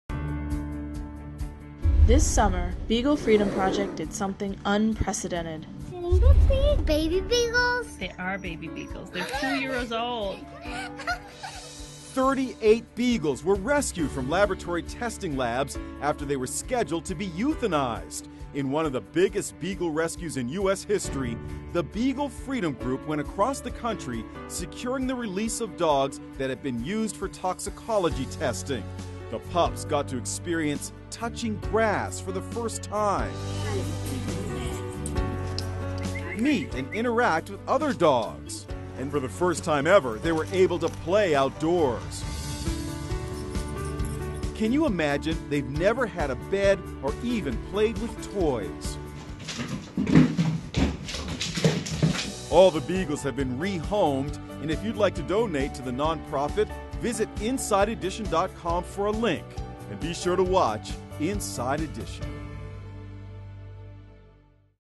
Beagles Step On Grass For First Time After Being Freed from Lab Testing
A moving video shows the moment 38 beagles felt freedom for the first time after spending their lives in cages. The pups, who were being used for toxicology testing at labs across five states, were saved by the Beagle Freedom Project.